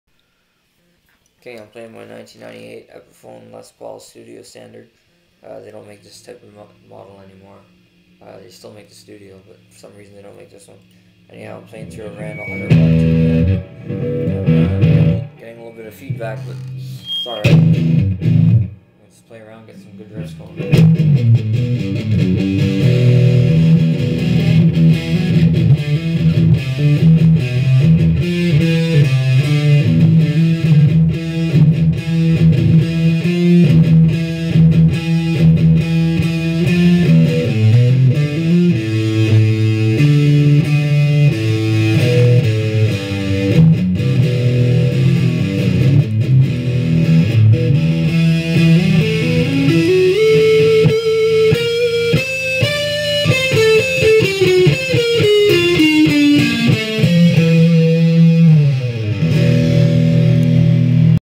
just messing around on my epiphone les paul playing some metal style riffs
the title says it all. i dont know scales or modes or any of that. i just play whatever i feel like playing. maybe its beautiful to someones ears or just noise. so if someone hates on me for this. ill say can you do better? this was just an easy riffing video. im capable of much more intricate things